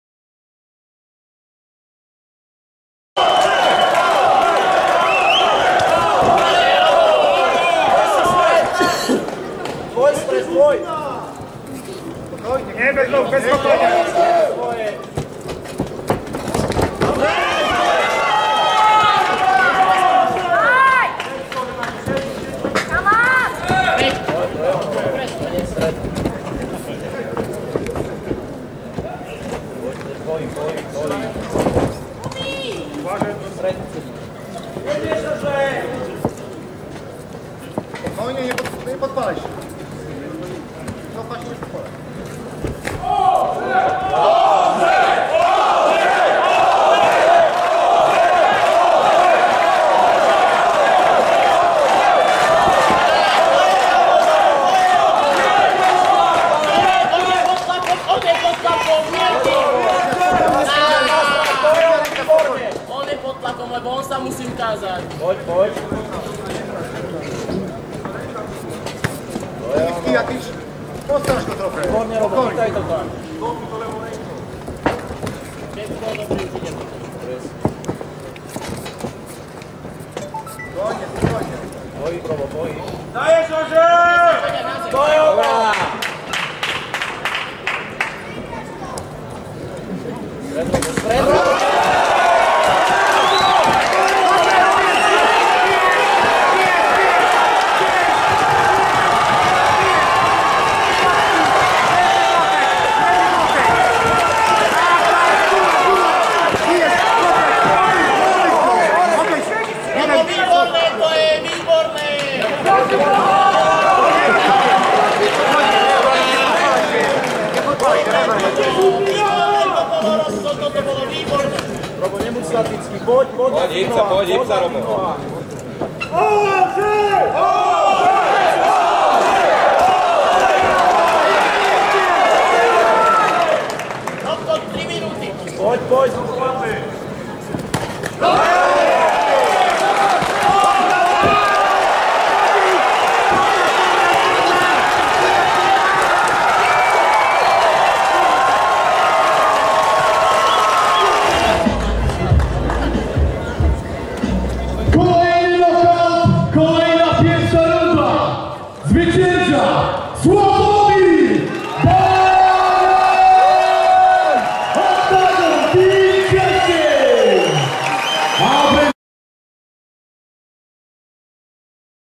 Szarża MMA 1: Sławomir Baran vs. Robert Delič [NOKAUT]
Nagranie walki zawodowej z gali Szarża MMA 1: Sławomir Baran vs. Robert Delič